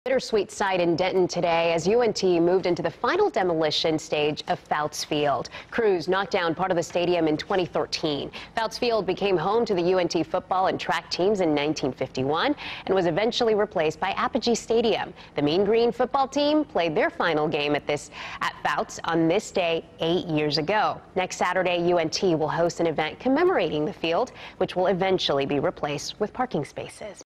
UNT Begins Demolishing Rest Of Fouts Field
The Mean Green football team played its final game at Fouts Field on this day, eight years ago.